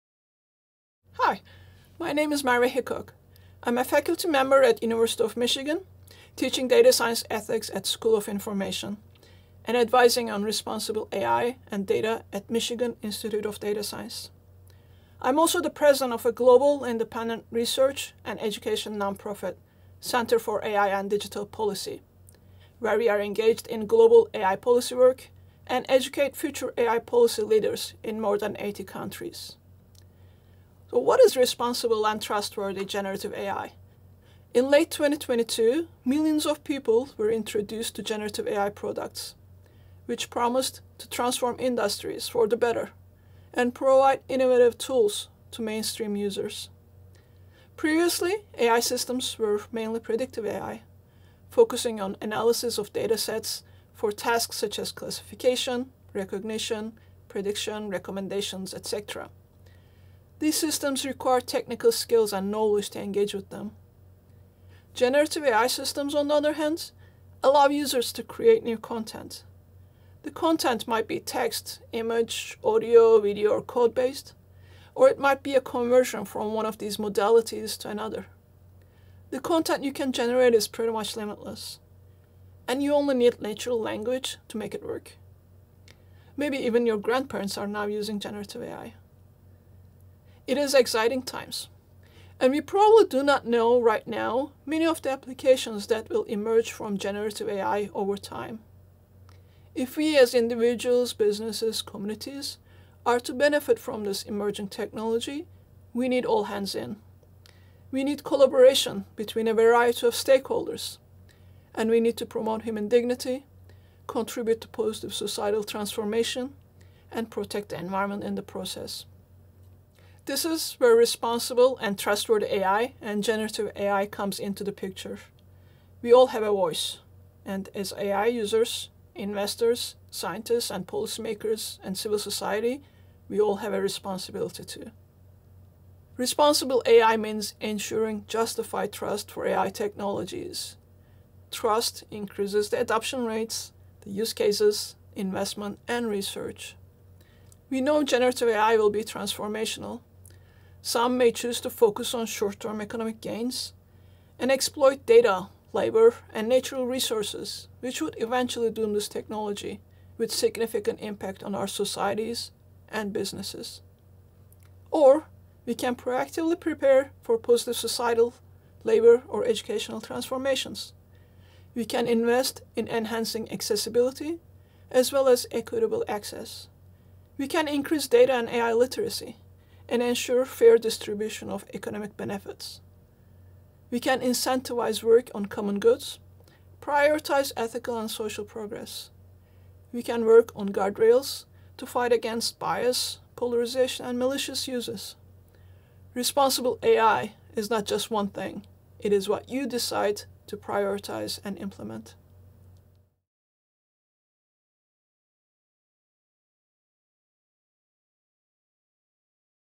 Responsible and Trustworthy Generative AI
Merve Hickok, a renowned expert on AI policy, ethics, and governance at the University of Michigan School of Information, talks about what responsible and trustworthy generative AI would look like in this fast-evolving landscape.

With seemingly limitless applications of the technology, Hickok argues that there must be trust in the technology for it to truly benefit our societies. We can invest in accessibility, equitable access, AI literacy, prioritize ethical and social progress, fight against bias, polarization, and malicious use. “We all have a voice, … and we all have a responsibility too.”

Learn more about generative AI in the short online course “Generative AI Essentials: Overview and Impact” now available on Michigan Online.